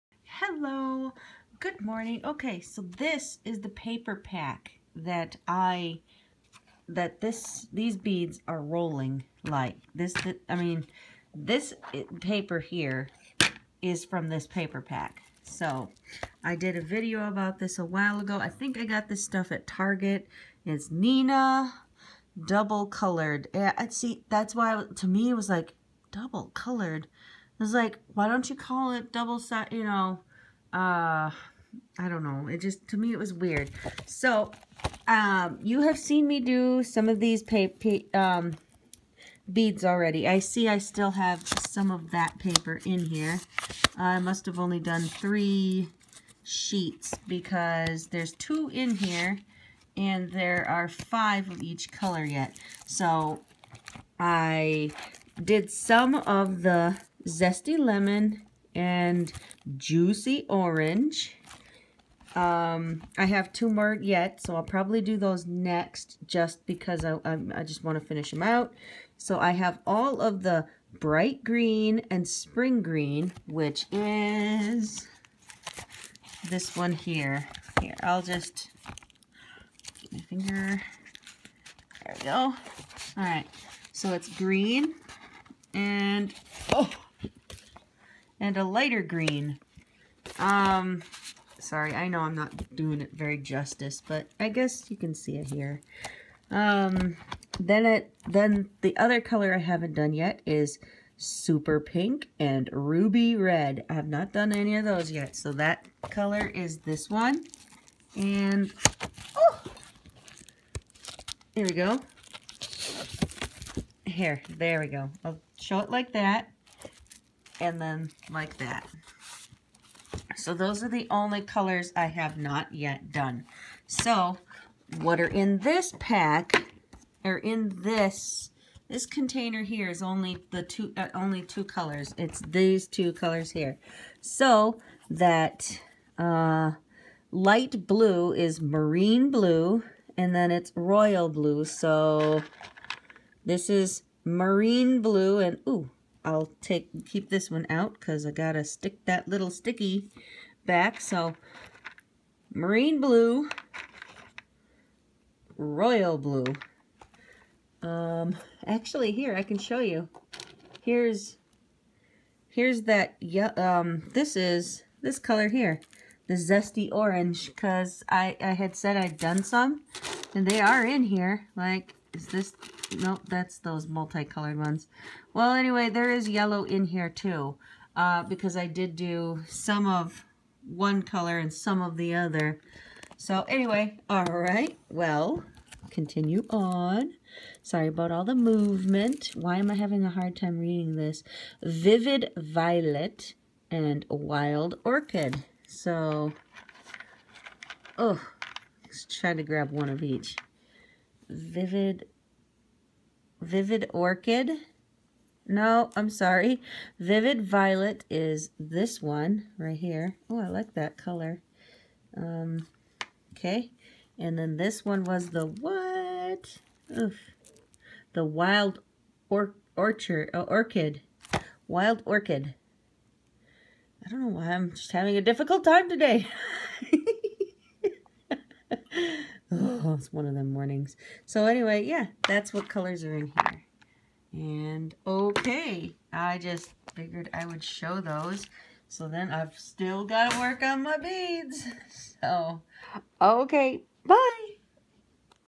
The paper pack I am using
This video I'm just showing the paper pack and color names for the most recent rolled paper beads.  Enjoy!